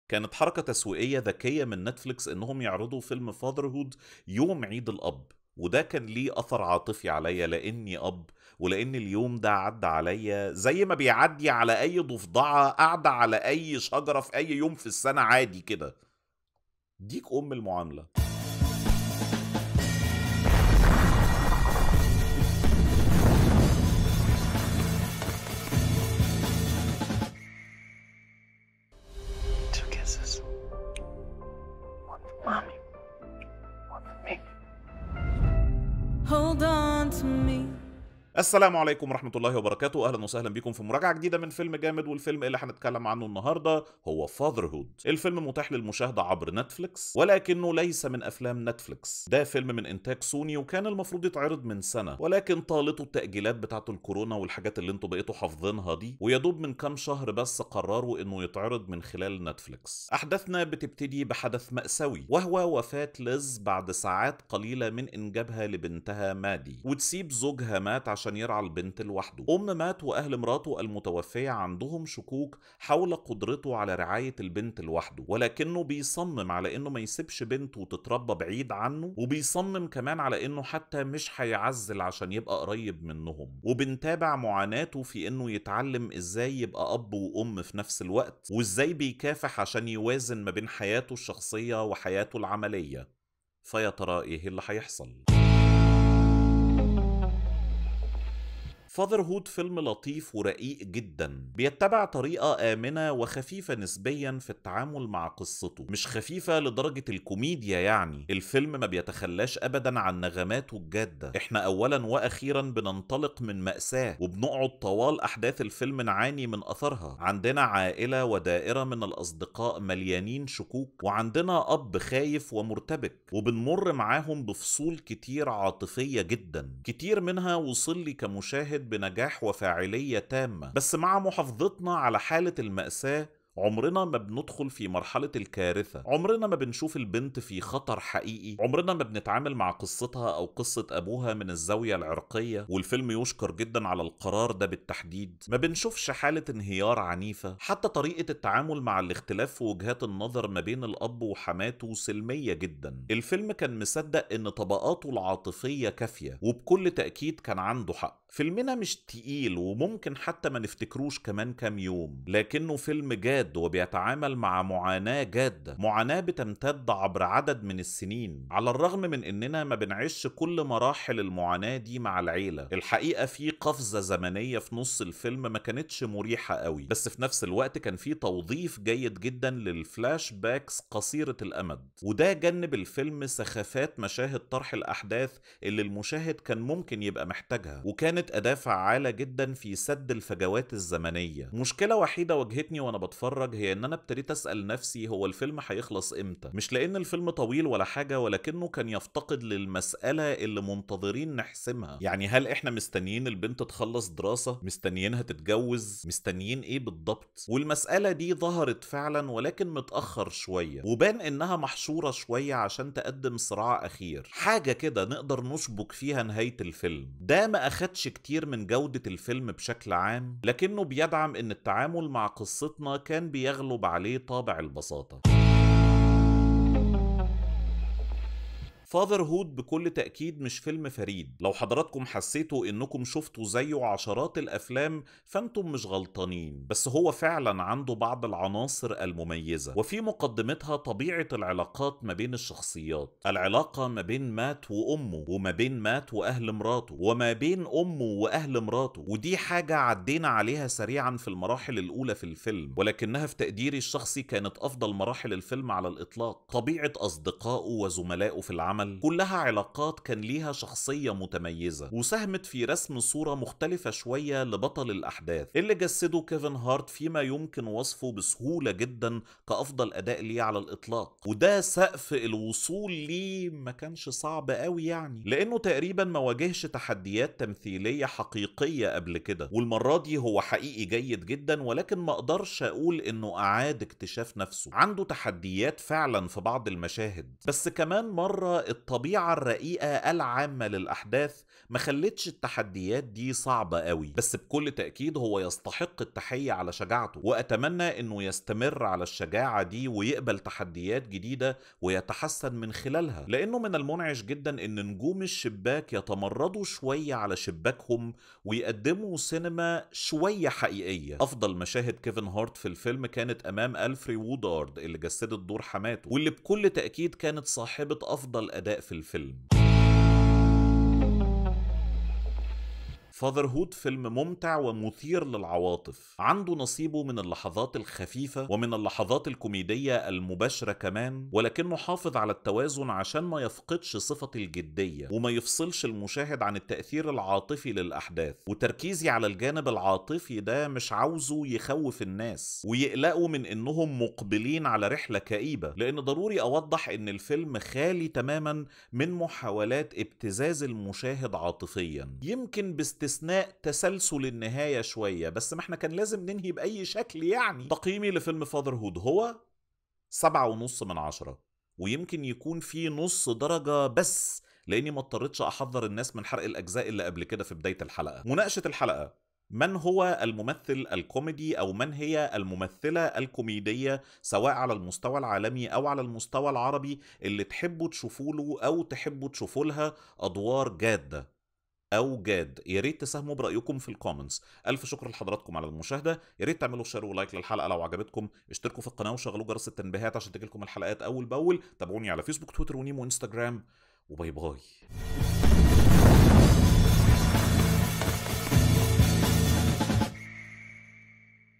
مراجعة فيلم "Fatherhood" بدون حرق | FilmGamed
مراجعة فيلم Fatherhood من فيلم_جامد بدون حرق للأحداث. الفيلم هو أحدث إصدارات نتفليكس ن إنتاج سوني والأحداث تدور في إطار عائلي حول معاناة رجل ليملأ فراغ الأم في حياة طفلته حديثة الولادة. الفيلم من بطولة كيفن هارت وألفري وودارد وبول ريزر وليل ريل هاوري. السيناريو مأخوذ عن مذكرات مات لوجلين على يد دانا ستيفنز وبول فايتز الذي أخرج الفيلم كذلك.